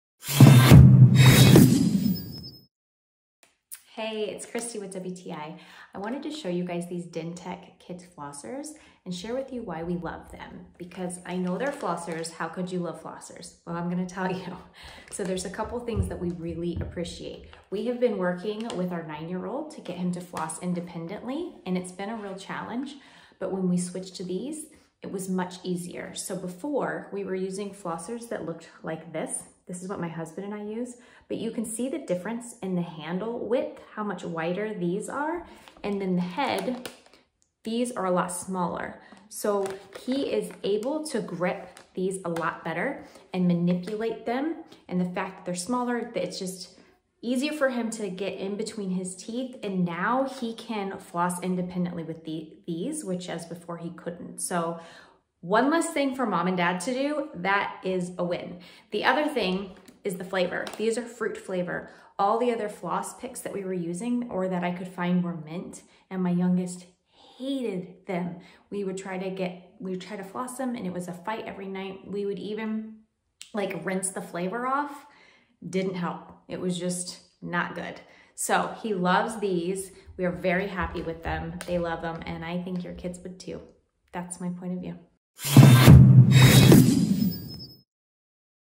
Our Point of View on DenTek Kids Fun Flossers From Amazon
(Commissionable Links)

about this PRODUCT:
75-Count per pack. 2-Packs total DenTek helps you encourage flossing at an early age.
Small flosser head fits children’s mouths just right.
Easy-to-grip handle tailored for child-size hands
Wild fruit flavor kids love. Advanced fluoride coating.